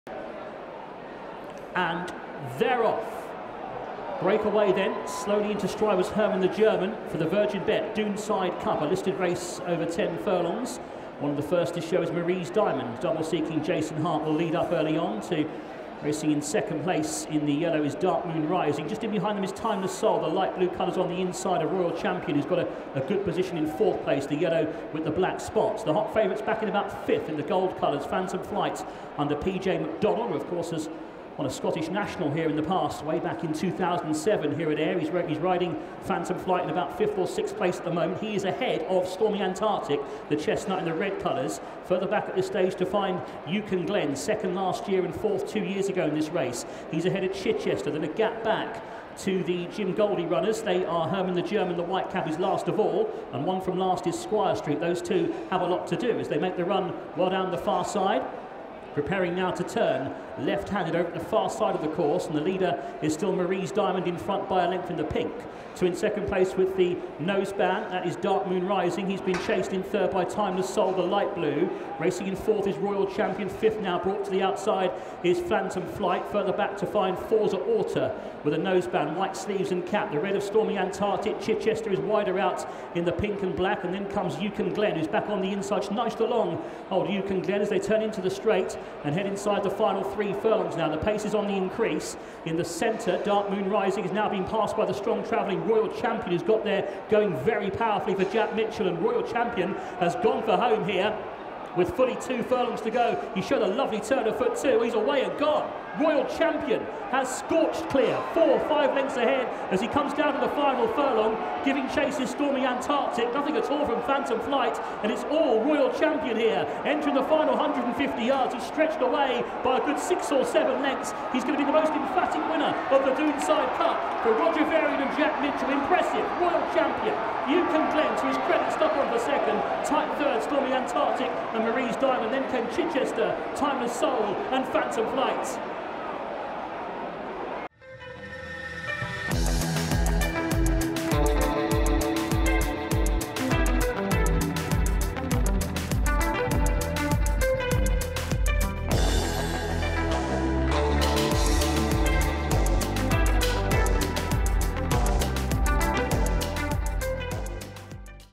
Seriously impressive! ROYAL CHAMPION bolts up in the Doonside Cup at Ayr
Roger Varian’s charge got back to winning ways in style under Jack Mitchell, benefitting from a drop in class in the Listed Virgin Bet Doonside Cup Stakes.